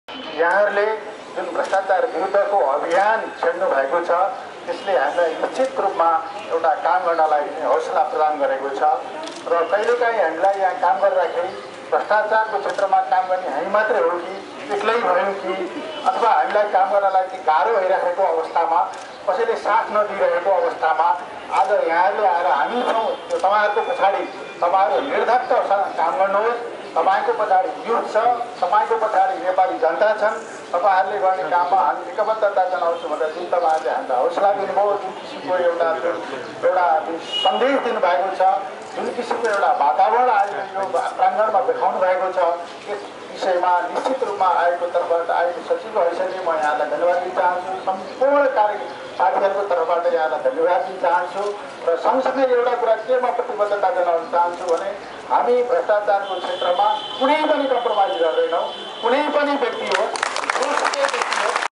Nepal unites with Bhagwati Kafle & CIAA against corruption
Nepal unites with Bhagwati Kafle and CIAA against corruption - भ्रष्टाचारीलाई किरा परोस्
अख्तियार दुरुपयोग अनुसन्धान आयोगका सचिव भगवती काफ्ले भ्रष्टाचार विरुद्ध प्रतिवद्धता जनाउँदै भ्रष्टाचार बिरुद्द हामी नेपाली एक हुनमा आवहान गर्दै। Bhagwati Kafle and Commission of investigation of abuse of Authority thanking Nepalis supporting actions against corruption and pledging to be tough on corruption from public officials in Nepal.
Maha jodi were there with hundreds of Nepali citizens to support Good people fighting corruption in Nepal